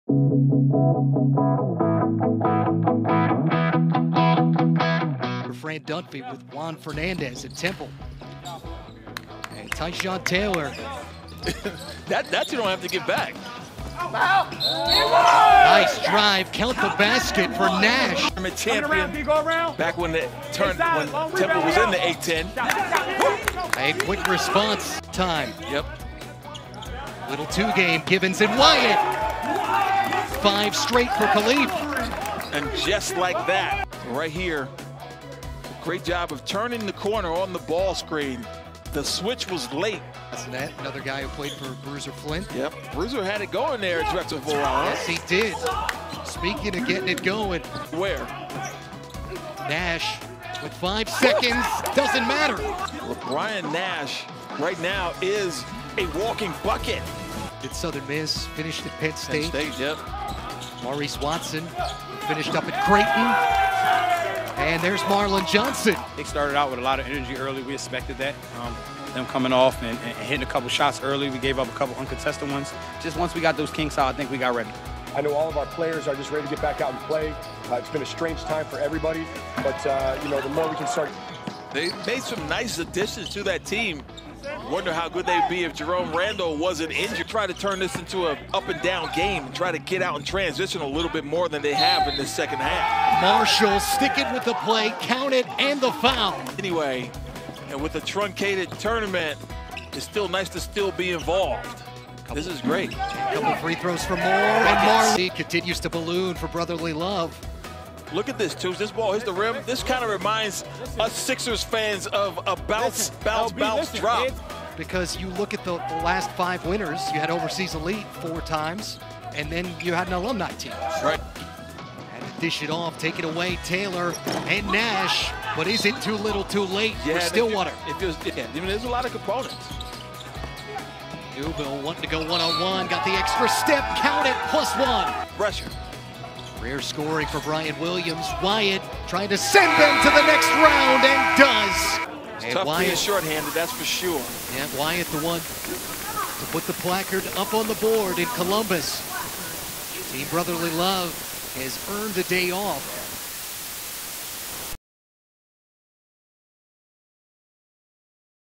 TBT 2020 #12 Brotherly Love vs #21 Stillwater Stars Highlights
Khalif Wyatt and Ramone Moore were a formidable backcourt duo at Temple University nearly a decade ago. In a first-round win over the Stillwater Stars, the two guards came together once again.

Wyatt led Brotherly Love with 27 points on 7-for-10 shooting as Brotherly Love overcame an early 10-point deficit and defeated the Stillwater Stars, 87-71.

“We played together in college,” said Wyatt. “We were a good 1-2 punch [then]. [Moore] takes pressure off of me.”

Moore had 24 points in the win for Brotherly Love, which shot 92% (22-for-24) from the free-throw line.

Le’Bryan Nash led all scorers with 30 points on 12-of-18 shooting in the losing effort for the Stars, who were outrebounded 38-26.

“[Nash] was great obviously,” said Stars coach Bryndon Manzer. “A lot was put on his shoulders, and he delivered.”

“It was a tough battle for us,” said Manzer. “We hung tough for a long time.”

Brotherly Love advances to face Eberlein Drive on July 6th at 7:00 PM EST.

TBT features 24 teams of top professional, college alumni, and international basketball players competing in a single-elimination 5-on-5 tournament for a winner-take-all $1 Million prize. All games will air on ESPN networks beginning Saturday, July 4.